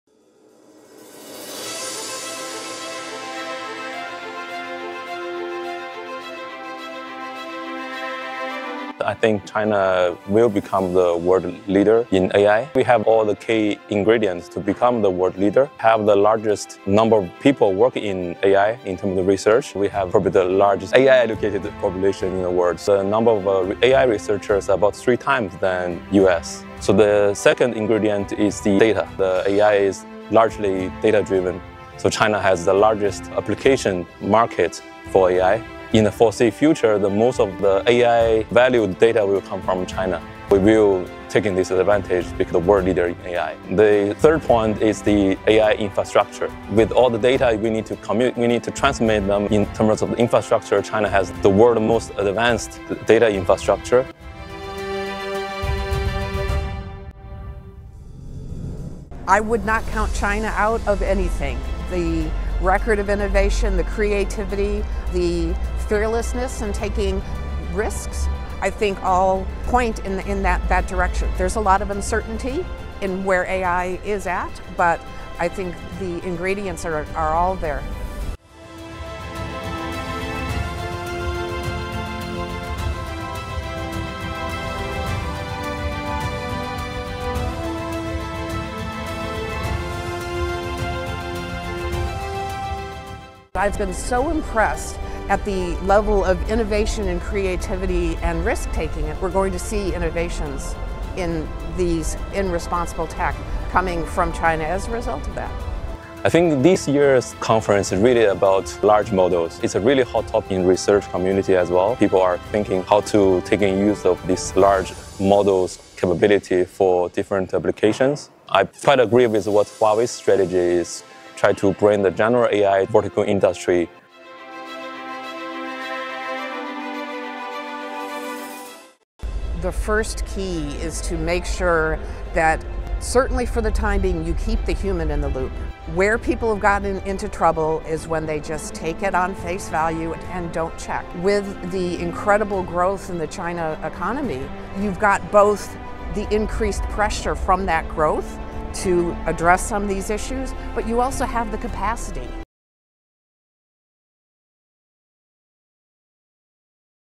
GLOBALink | China set to be AI leader by 2030: sector insiders from home and abroad
"I think (in 2030) China will become the world leader in AI," said an expert at the Chinese Academy of Sciences, adding that we have all the ingredients to become the world's leader in this field.
"I would not count China out of anything," said Thoughtworks' CTO Emerita.